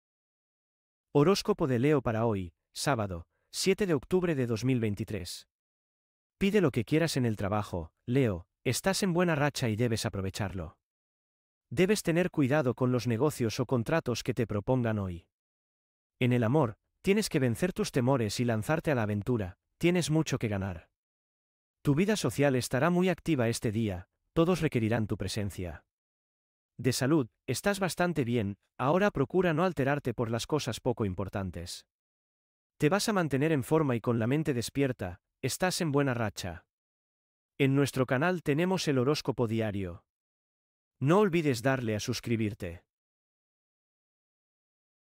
HORÓSCOPO DIARIO 💖❤️🌟 LEO ♌️ SÁBADO, 7 DE OCTUBRE DE 2023
Horóscopo de Leo para hoy, sábado 7 de Octubre de 2023. El horóscopo para hoy: descubre tu futuro gracias a mi horóscopo diario para ti.